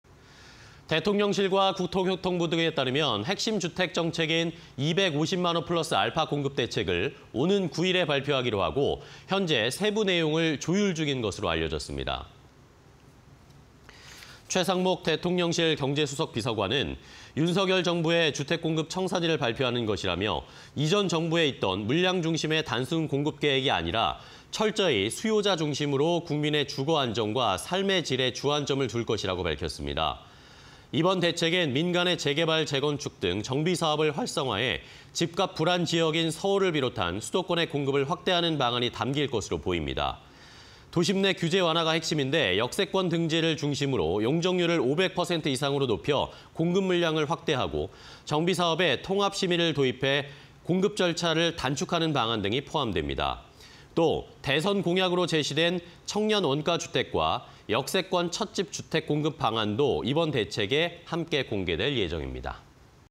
尹정부 첫 주택공급대책 '250만호+α' 9일 발표 (20220805)
대통령실과 국토교통부 등에 따르면 핵심 주택 정책인 '250만호+α' 공급대책을 오는 9일에 발표하기로 하고 현재 세부 내용을 조율 중인 것으로 알려졌습니다.
최상목 대통령실 경제수석비서관은 "윤석열 정부의 주택 공급 청사진을 발표하는 것"이라며 "이전 정부에 있던 물량 중심의 단순 공급계획이 아니라 철저히 수요자 중심으로 국민의 주거안정과 삶의 질에 주안점을 둘 것"이라고 밝혔습니다.
이번 대책엔 민간의 재개발·재건축 등 정비사업을 활성화해 집값 불안 지역인 서울을 비롯한 수도권의 공급을 확대하는 방안이 담길 것으로 보입니다.
도심 내 규제완화가 핵심인데 역세권 등지를 중심으로 용적률을 500% 이상으로 높여 공급물량을 확대하고 정비사업에 '통합심의'를 도입해 공급 절차를 단축하는 방안 등이 포함됩니다.
대선 공약으로 제시된 청년 원가 주택과 역세권 첫 집 주택 공급 방안도 이번 대책에서 함께 공개됩니다.